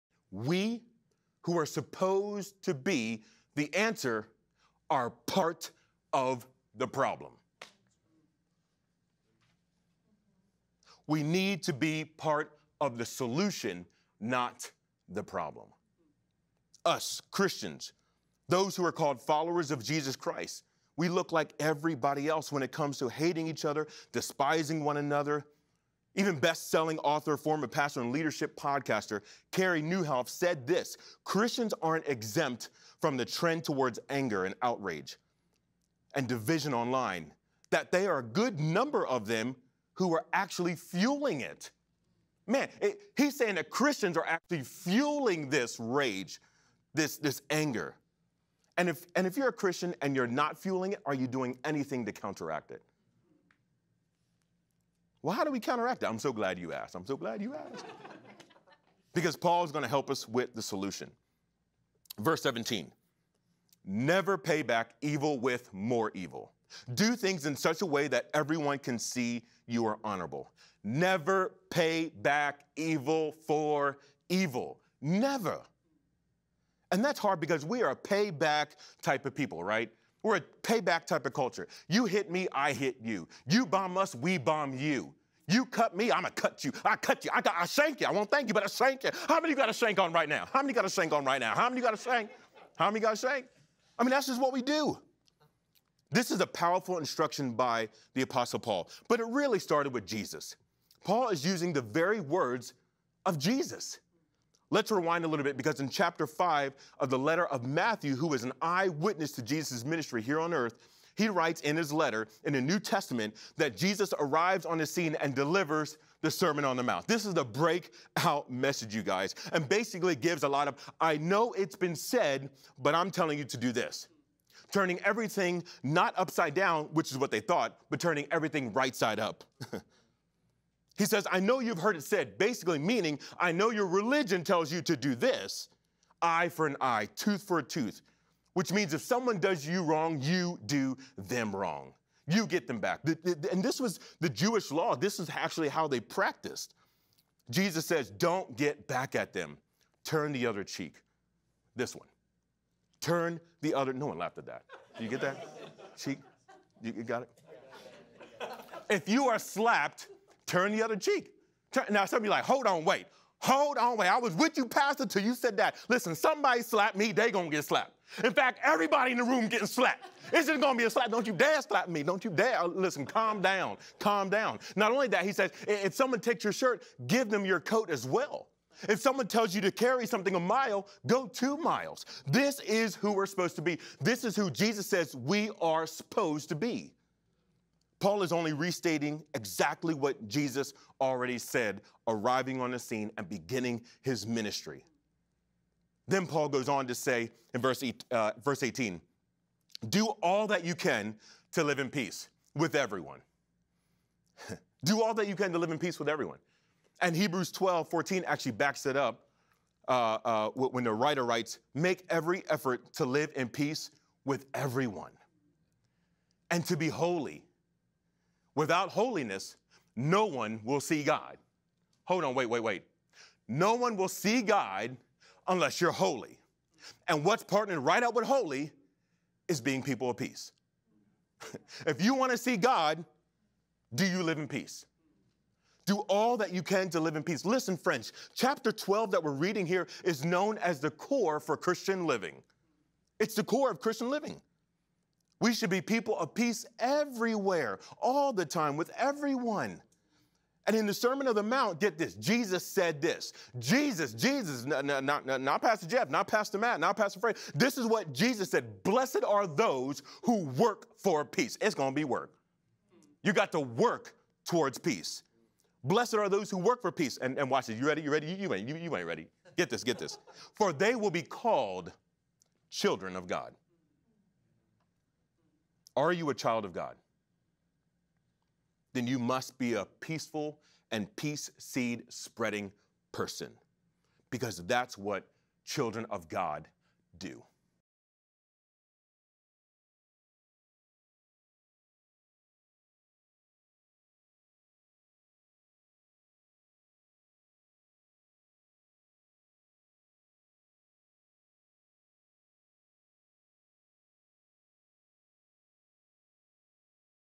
Christian...Are You Making the World Better? | Sandals Church
We live in an outrage culture...and a lot of Christians are contributing to the hate. What would Jesus do?

or use the Sandals Church App